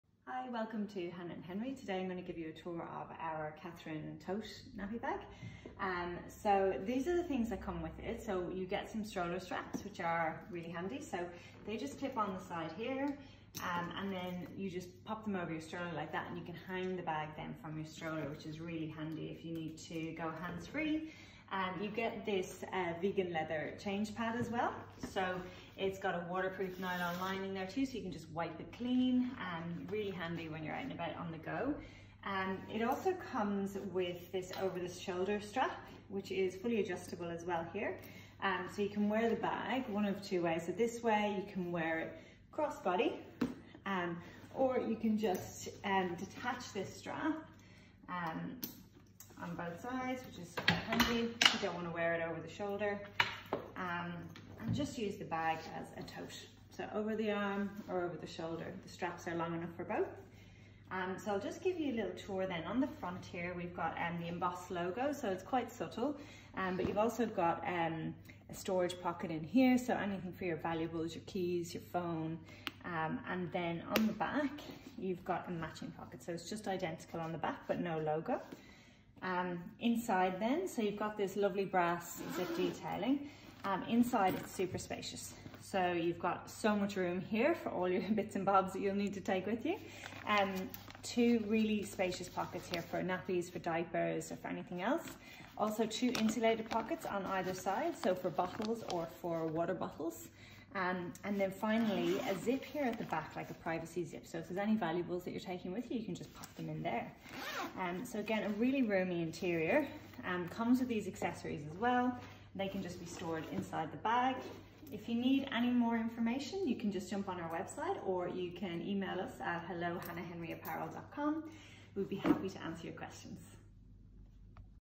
Catherine Carryall Tote Tan. Convertible Diaper Bag. Stylish Baby Bag. Vegan Leather Nappy Backpack.
The Catherine Carryall Tote is not your typical nappy bag, it's simple design features and sleek contouring gives this bag a sophisticated look. The Catherine Carryall Tote is the perfect companion for any busy Mum, whether you’re going to work, the shops, the gym or taking the kids to the park! Crafted using only the finest vegan leather on the exterior and durable nylon lining for the interior.

Why we love the Catherine Carryall Tote:

The Catherine Carryall Tote has 7 compartments in total

An extra-large main compartment for storage

There is 1 privacy pocket with zip for your valuables

There are 2 large interior pockets made from water-resistant nylon

There are 2 Insulated interior pockets that can be used for baby bottles

A longer shoulder strap included so you can wear your Tote crossbody

Stroller straps included for when you need your hands free

Both interior and exterior are spill proof and wipeable

Measurements:

Diaper Bag: 30.5cm (w) x 28cm (h) x 20cm (d)

Changing Mat: 12cm (w) x 23.5cm (L)

Strap Drop: 20cm

Shoulder Strap Length: 127cm

Weight: 1.3kg